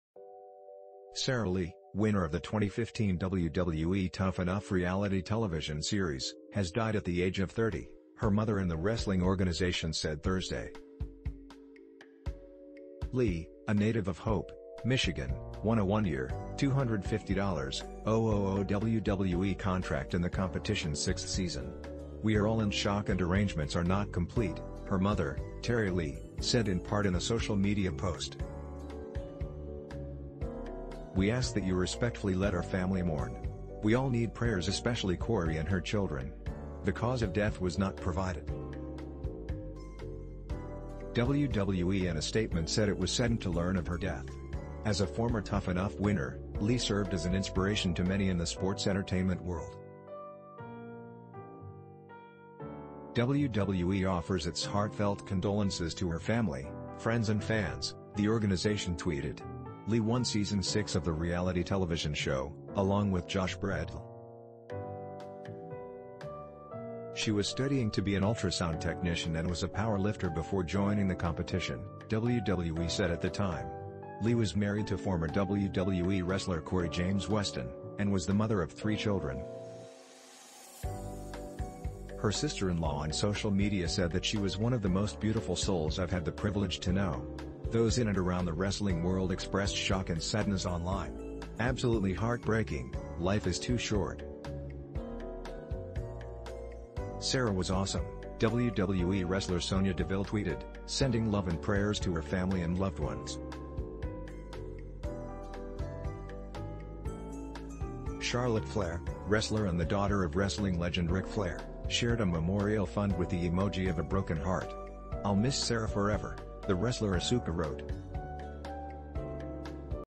Sara Lee former WWE ‘Tough Enough’ winner dies at 30
Sara Lee, winner of the 2015 WWE "Tough Enough" reality television series, has died at the age of 30, her mother and the wrestling organization said Thursday. Lee, a native of Hope, Michigan, won a one-year, $250, 000 WWE contract in the competition's sixth season."We are all in shock and arrangements are not complete, " her mother, Terri Lee, said in part in a social media post. "We ask that you respectfully let our family mourn. We all need prayers especially Cory and her children. "The cause of death was not provided. WWE in a statement said it was saddened to learn of her death.“As a former “Tough Enough” winner, Lee served as an inspiration to many in the sports-entertainment world. WWE offers its heartfelt condolences to her family, friends and fans, ” the organization tweeted. Lee won Season 6 of the reality television show, along with Josh Bredl. She was studying to be an ultrasound technician and was a power lifter before joining the competition, WWE said at the time. Lee was married to former WWE wrestler Cory James Weston, and was the mother of three children. Her sister-in-law on social media said that she was "one of the most beautiful souls I’ve had the privilege to know. "Those in and around the wrestling world expressed shock and sadness online.“Absolutely heart breaking, life is too short. Sara was awesome, " WWE wrestler Sonya Deville tweeted, “sending love and prayers to her family and loved ones. ”Charlotte Flair, wrestler and the daughter of wrestling legend Ric Flair, shared a memorial fund with the emoji of a broken heart.“I’ll miss Sara forever, " the wrestler Asuka wrote.